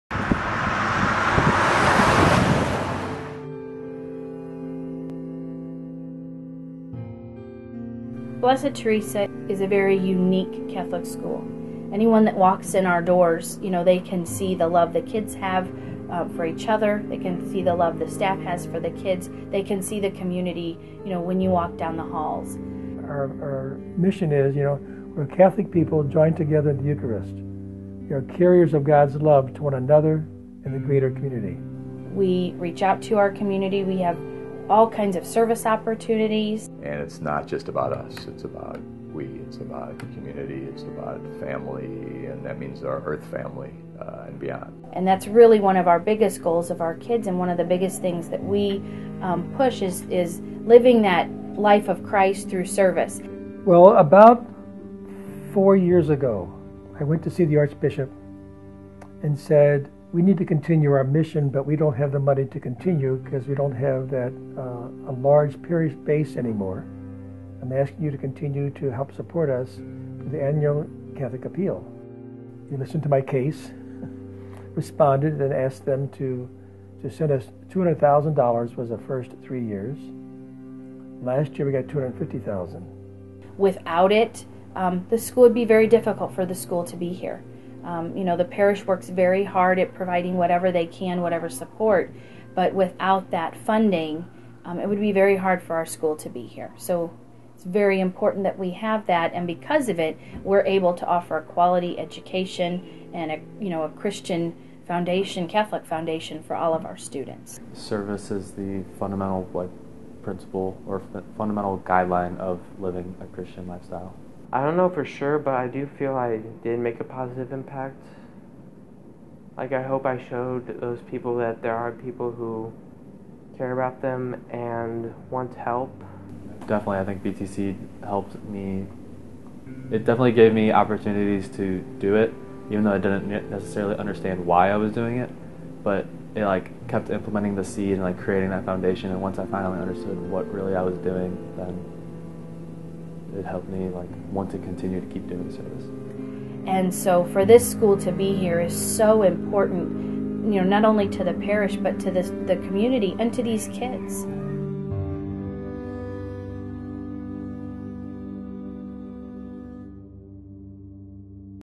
Archdiocesan Annual Catholic Appeal Video 1 - De Smet Jesuit 2017
Video created by Luis Mejia '17 & Andrew Bourbon '19 of De Smet Jesuit High School for the Annual Catholic Appeal of the Archdiocese of St. Louis.  The video is based on the work of Blessed Teresa of Calcutta Parish.

Thank you to Blessed Teresa of Calcutta School and Parish for their time and kindness.

Filming, directing, and editing: Luis Mejia Ahrens '17 and Andrew Bourbon '19